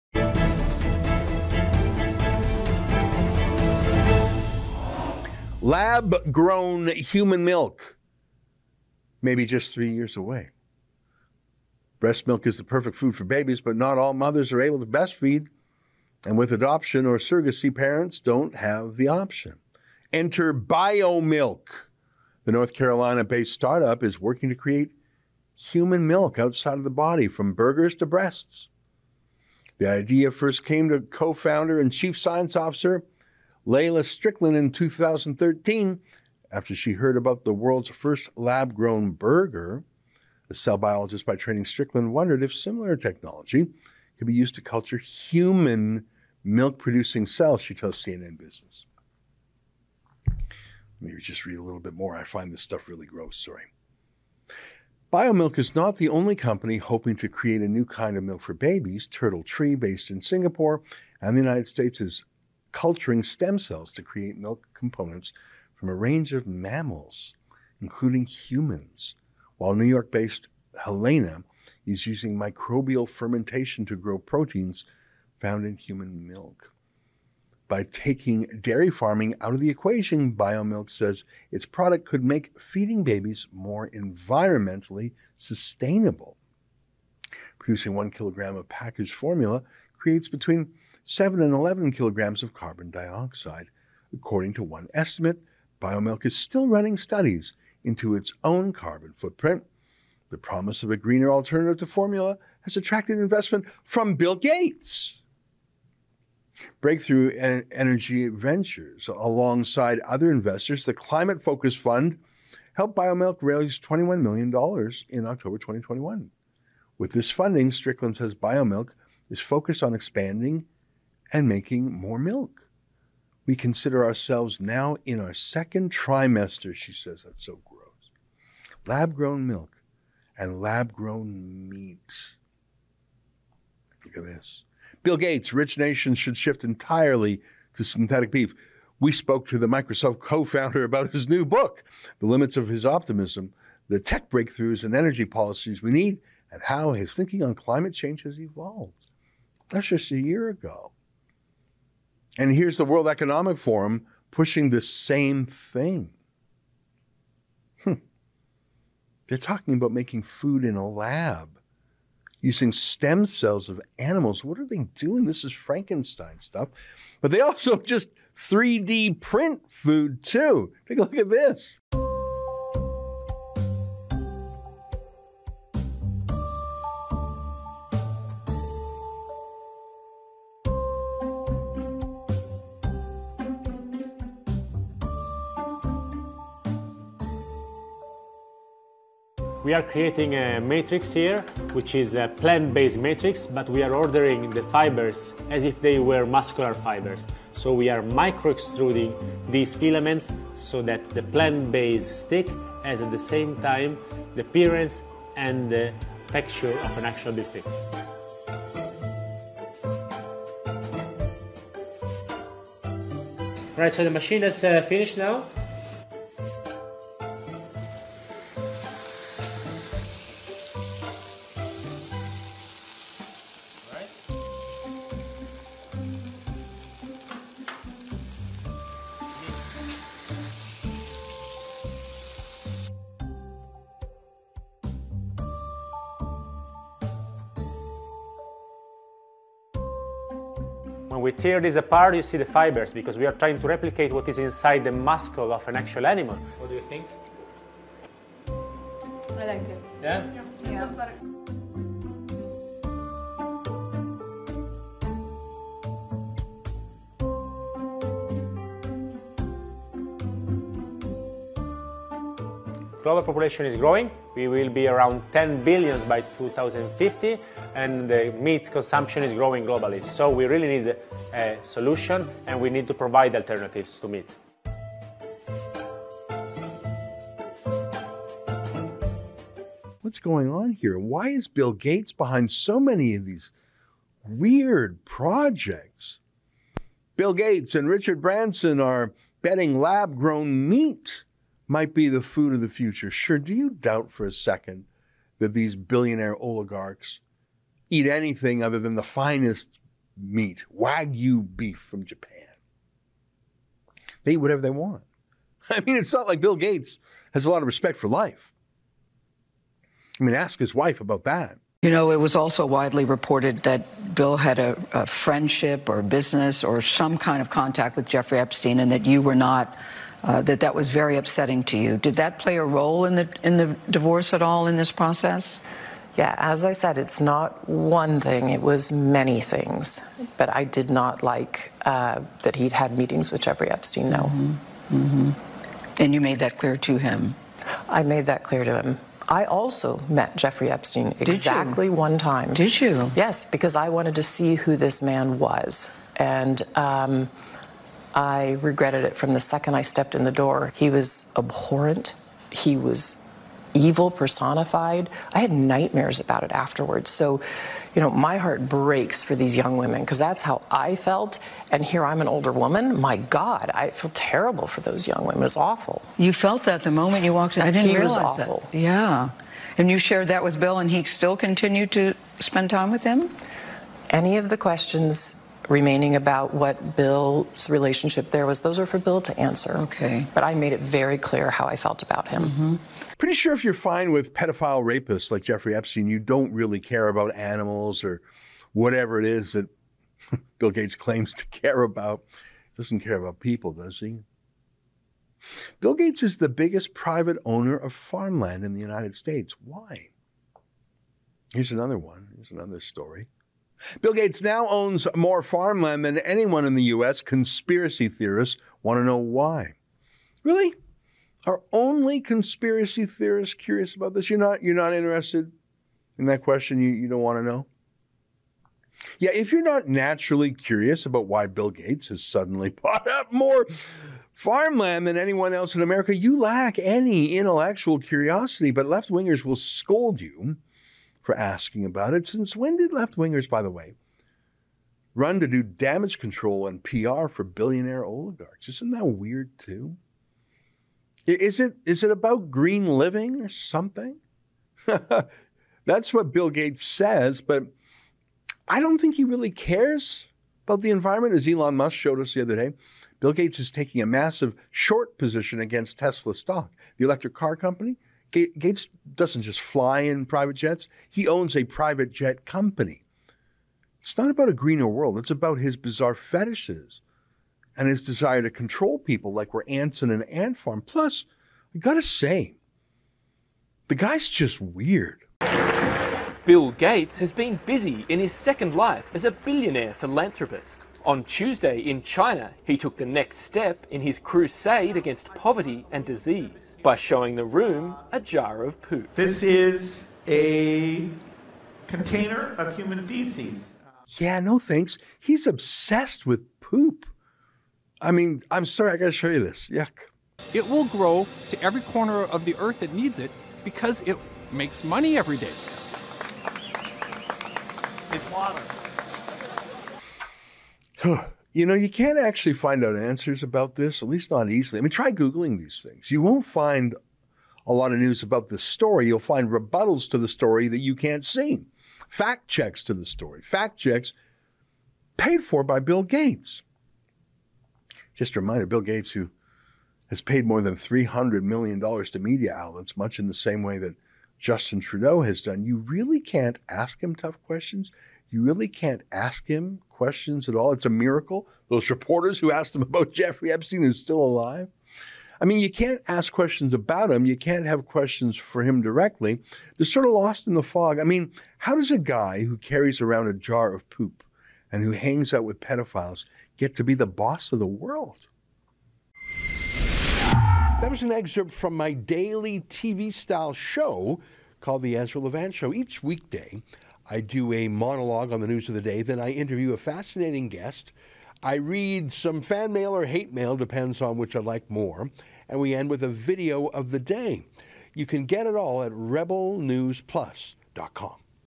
Bill Gates pushes lab-grown 'human milk' & synthetic beef as pillars of green future
Billionaire and former Jeffrey Epstein companion Bill Gates is moving full-throttle towards his utopian vision of a green new world. Not only has Mr. Gates invested heavily in and preached in favor of using synthetic beef as an antidote to climate change, Mr. Gates now has his sights set on promoting artificial or lab-grown 'human milk' for babies to consume.